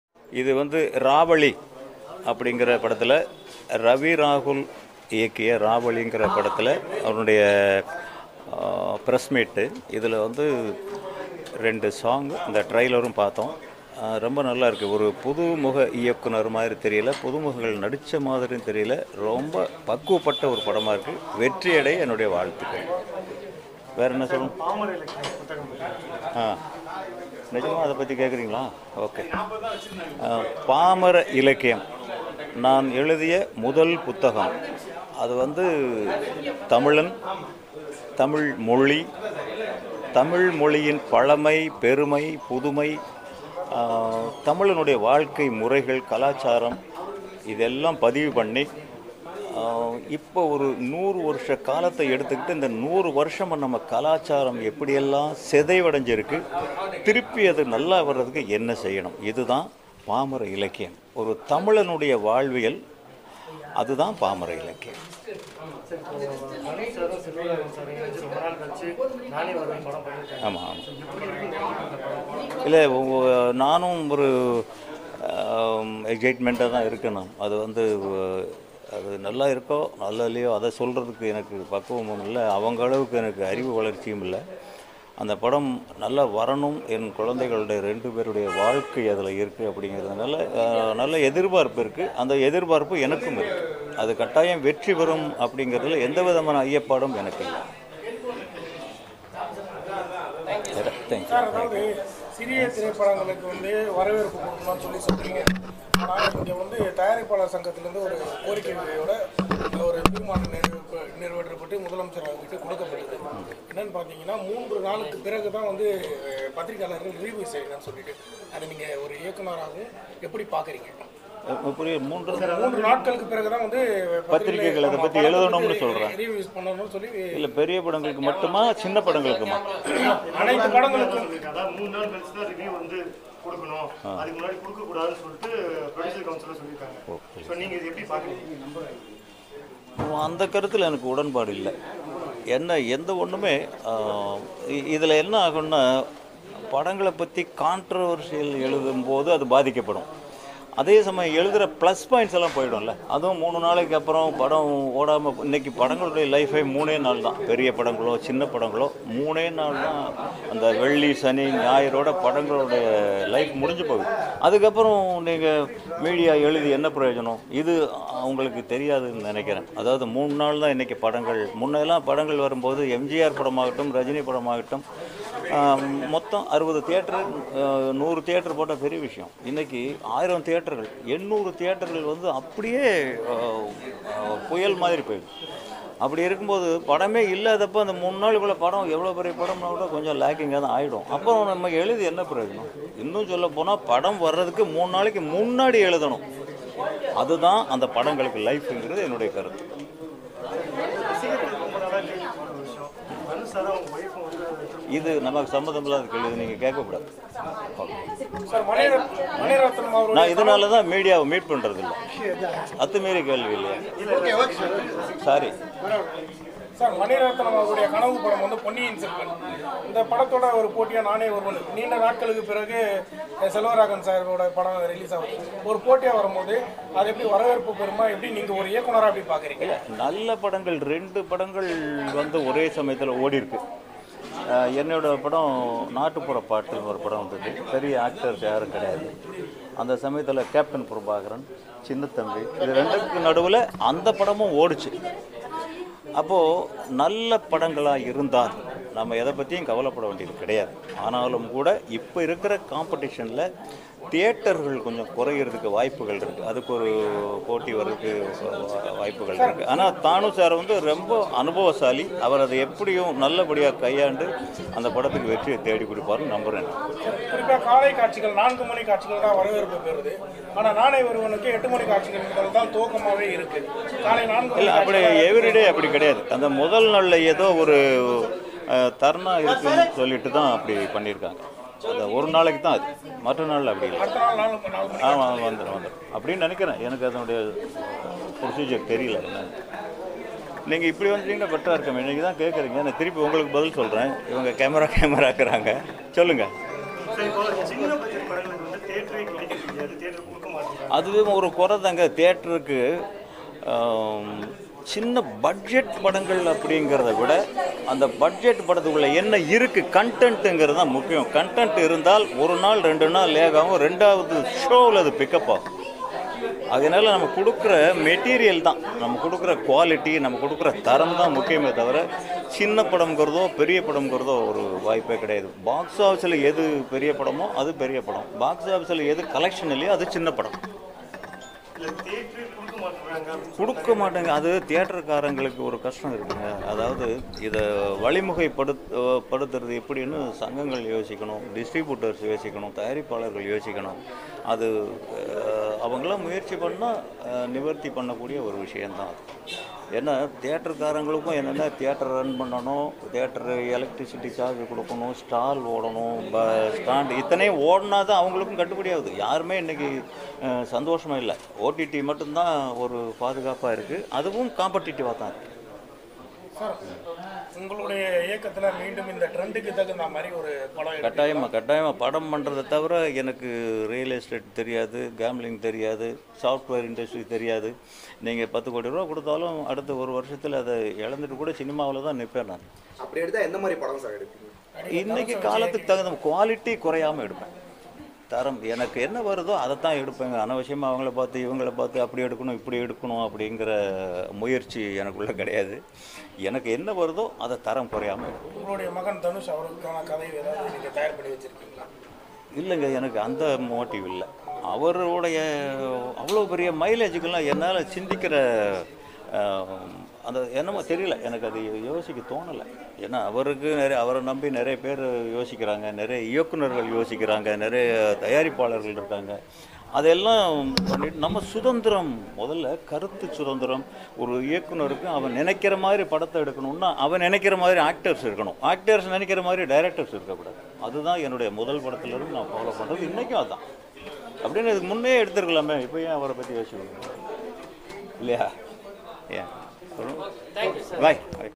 Dhanush - Aishwarya! கடுப்பான கஸ்தூரி ராஜா! Kashthuri Raja Angry Press Meet | Ravaale Audio Launch
ராவளி படத்தின் இசை வெளியீட்டு விழாவில் இயக்குனர் கஸ்தூரி ராஜா பேட்டி